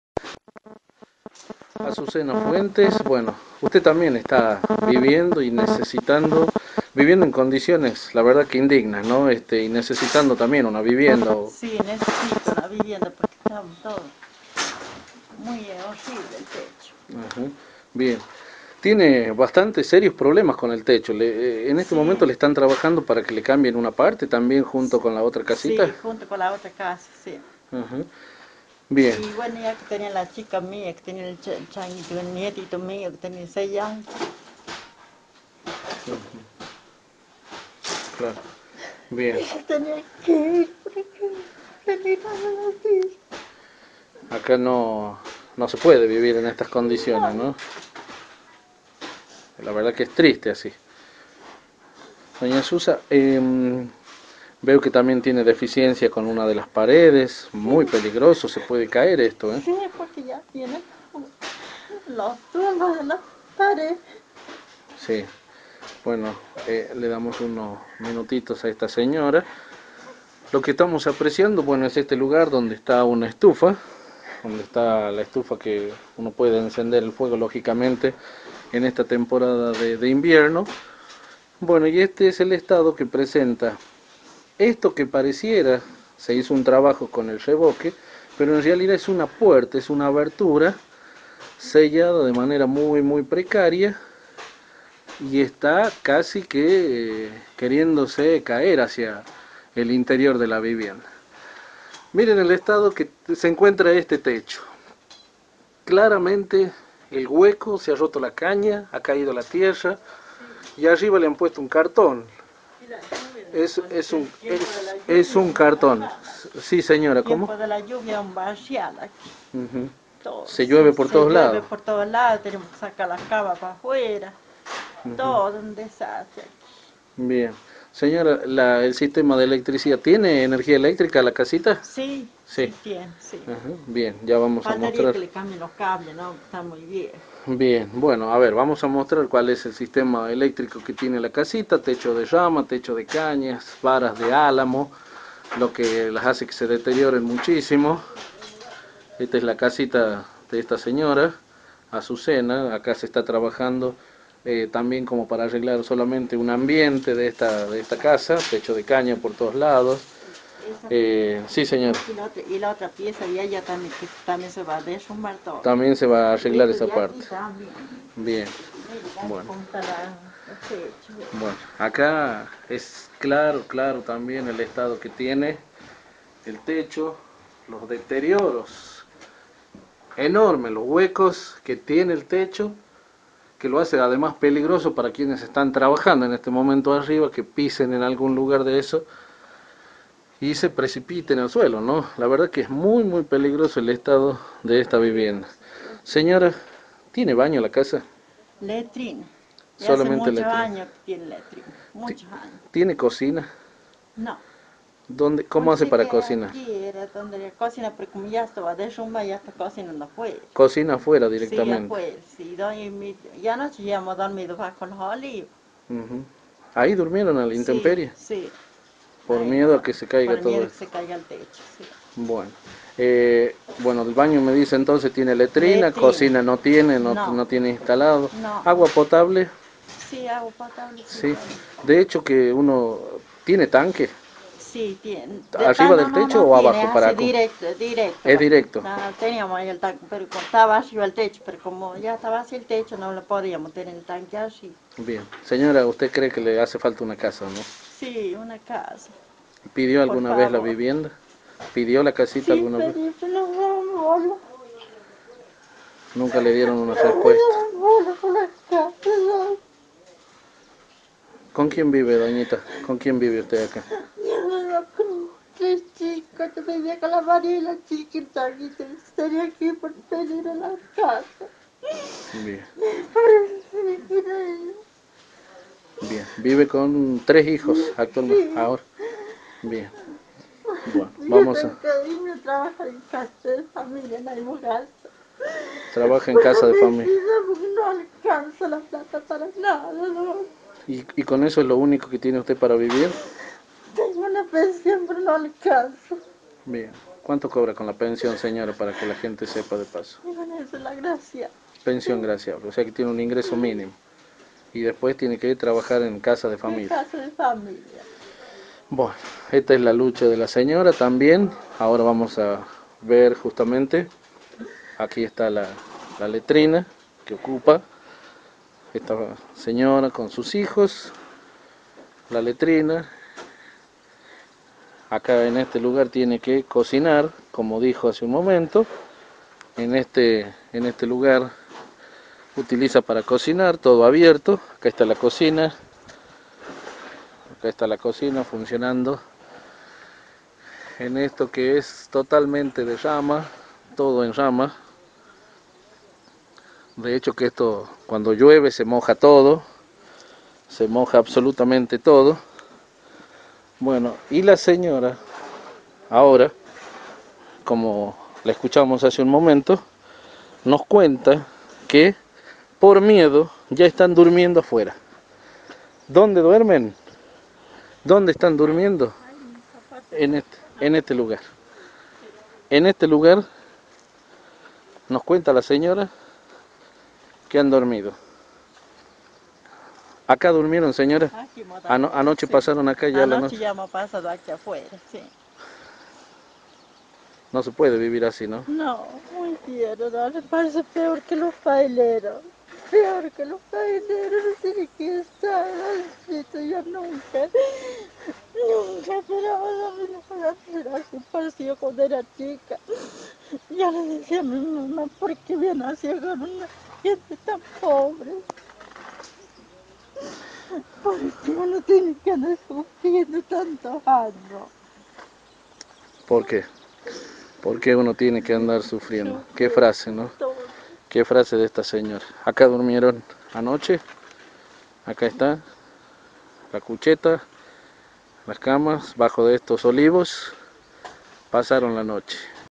Azucena Fuentes Muestra como vive en San Pedro 20 08 12
Tremenda situación habitacional de la señora, también en San Pedro.